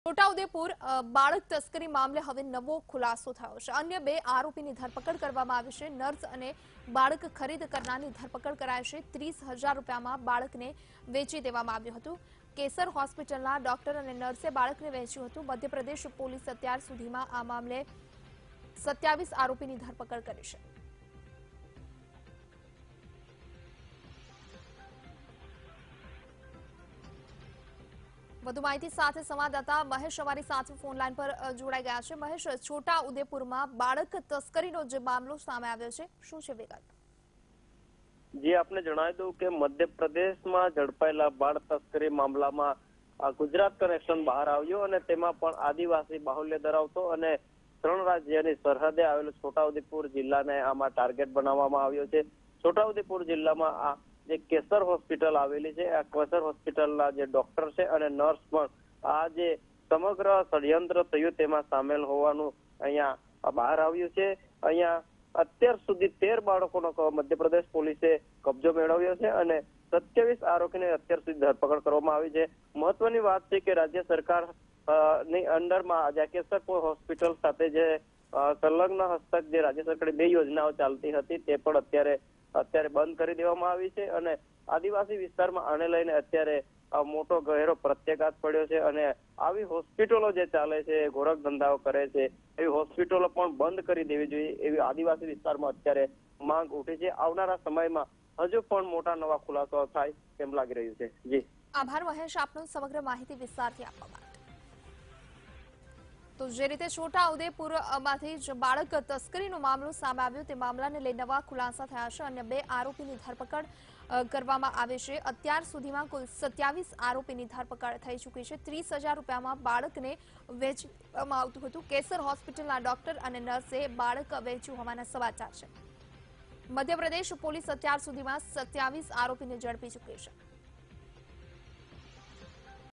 Chhota Udaipur: Human Trafficking Scam માં Police એ વધુ 2 આરોપી ની ધરપકડ કરી | Vtv News
Chhota Udaipur: Human Trafficking Scam માં Police એ વધુ 2 આરોપી ની ધરપકડ કરી.